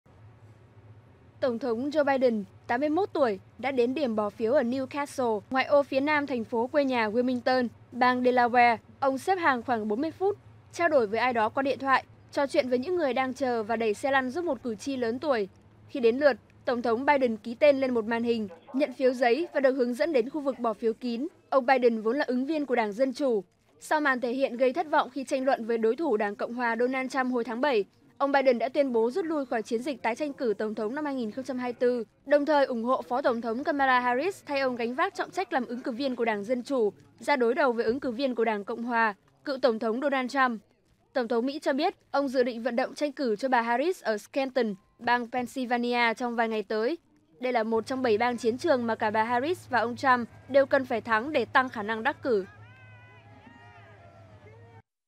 Tổng thống Mỹ Joe Biden đi bỏ phiếu sớm | Tin tức | Tin quốc tế
Tổng thống Joe Biden ngày 28/10 đã đi bầu cử sớm tại bang quê nhà Delaware (Đê-la-goe), khi còn một tuần nữa sẽ đến ngày bỏ phiếu chính thức.